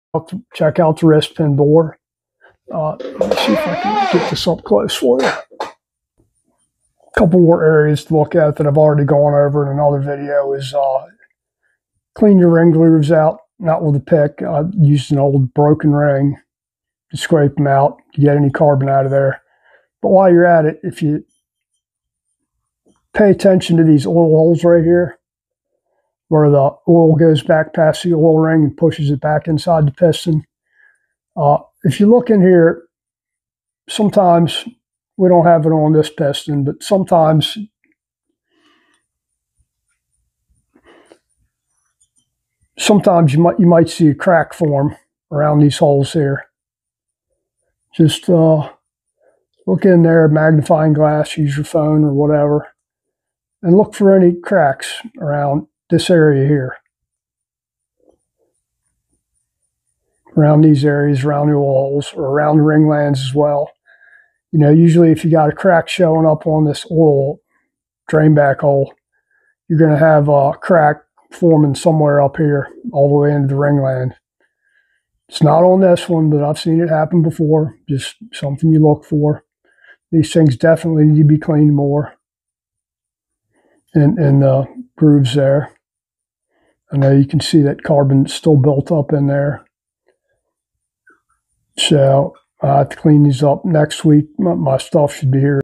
How To Spot Piston Problems Fast!
Taking care of your engine involves regular piston maintenance. This video demonstrates how to perform an inspection on a piston, paying special attention to the rings and piston pin. Follow these steps to keep your engine running smoothly and learn how to extend its life.